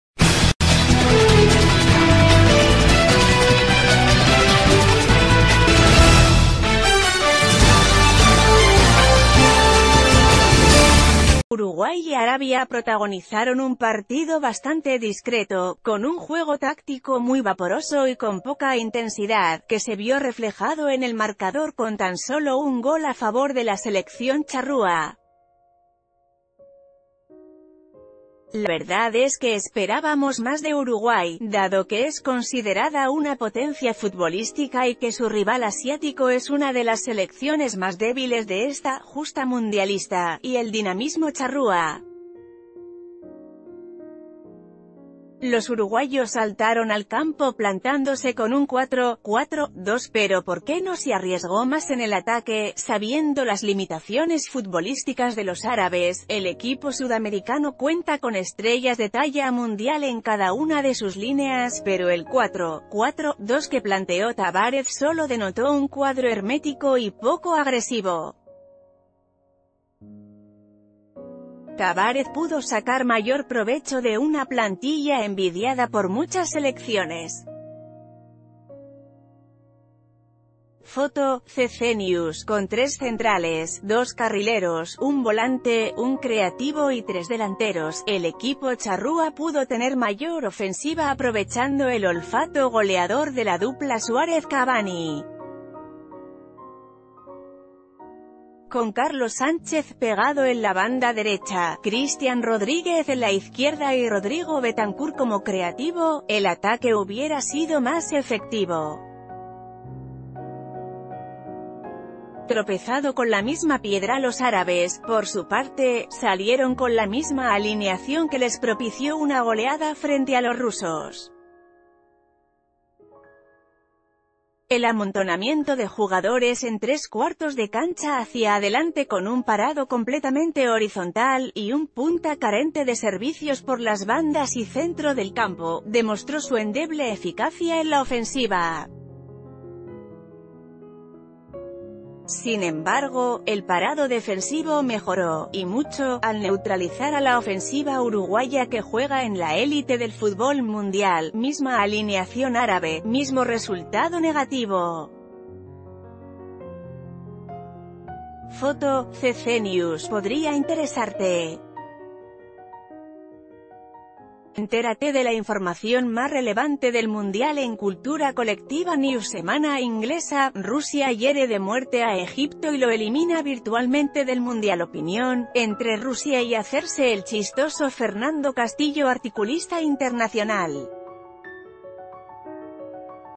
EL HUBIERA: ¿Por qué Uruguay no atacó más al enfrentar a una limitada Arabia?
Uruguay y Arabia protagonizaron un partido bastante discreto, con un juego táctico muy vaporoso y con poca intensidad, que se vio reflejado en el marcador con tan sólo un gol a favor de la selección charrúa. La verdad es que esperábamos más de Uruguay, dado que es considerada una potencia futbolísti...